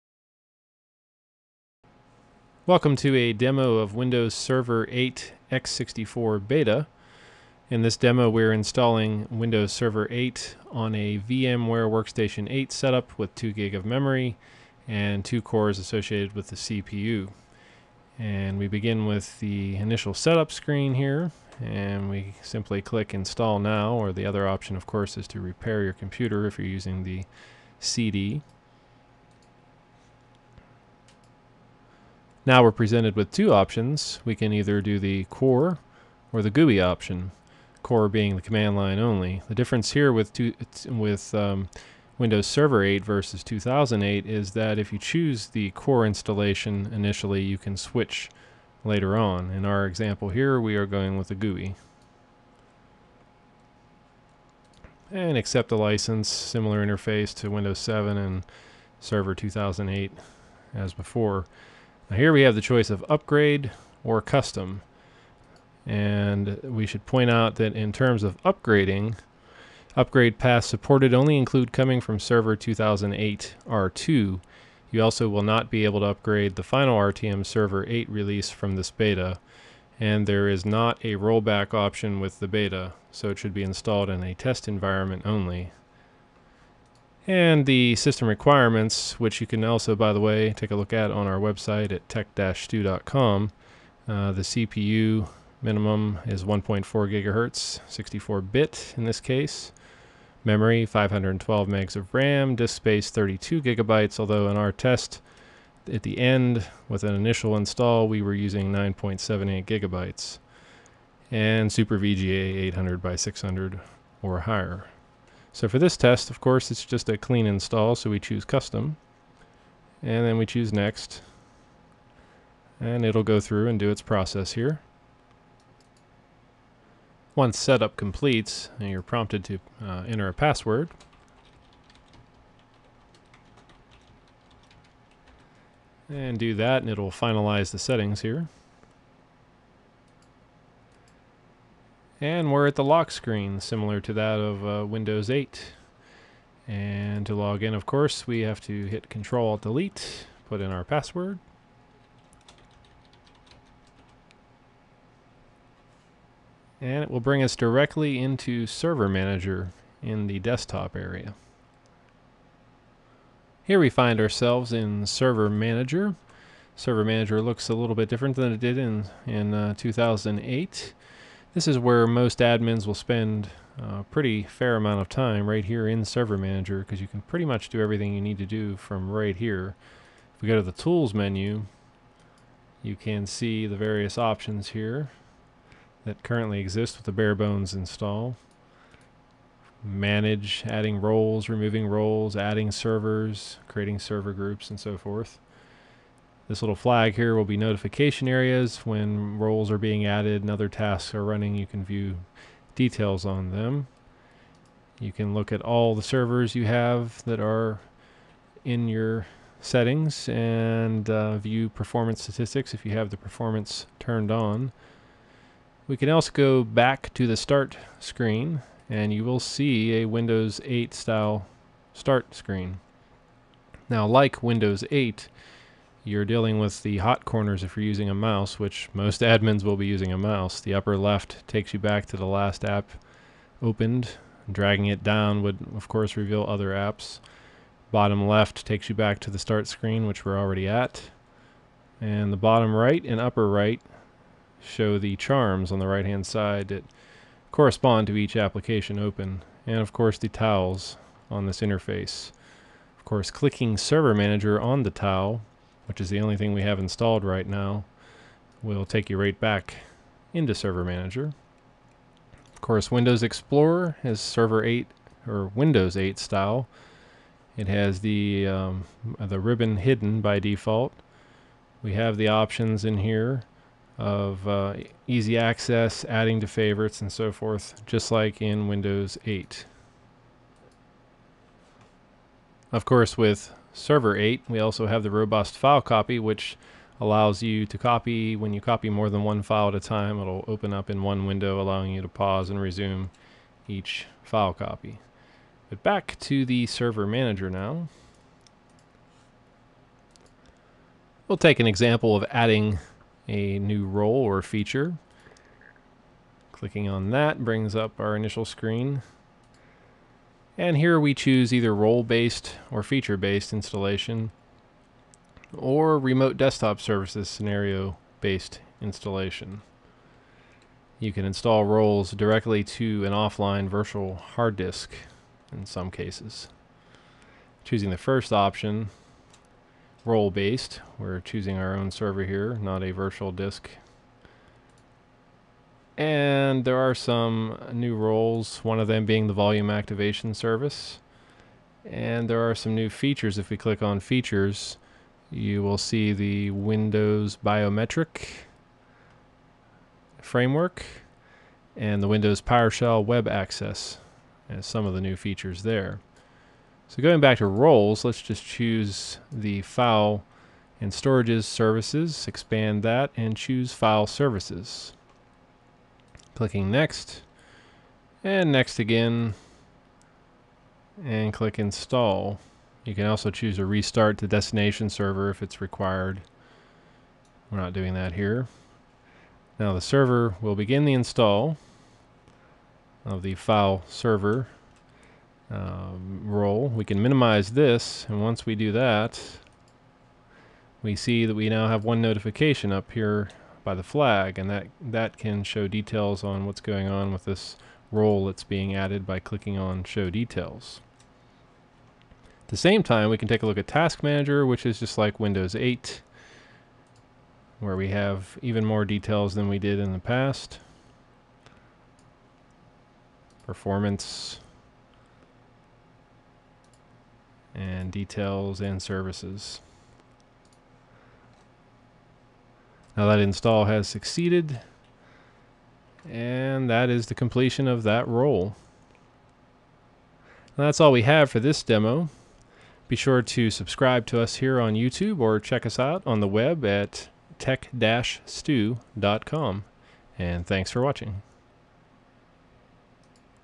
Windows Server 8 beta x64 Hands On, Overview + Installing Roles
Choose 720p for best quality.

As far as servers go, you don't think much of them.  They generally just sit in the background and do what they are supposed to.  One usually doesn't think of flashy GUI interfaces when it comes to servers either.  They generally are fairly straight forward and basic and in the case of Windows, haven't change a whole lot in the past several years, until now with Windows Server 8 and its Metro interface, which most likely will be largely ignored however, at least initially.

The Metro Tile interface has replaced the Start Button and Menu, just like in the Windows 8 Consumer Preview.  Microsoft is calling it the Metro-style interface.  There are tiles present that manage the functionality of the server.  You can even add more admin tiles by enabling them and subsequently you have the usual tools you are used to.

Since Server 8 has the same under the hood enhancements of Windows 8, it should be a better performer, compared to past server operating systems, at least to some degree.

I've put together this demo to show you some basic features of Server 8 beta and installing the File Services Role as an example.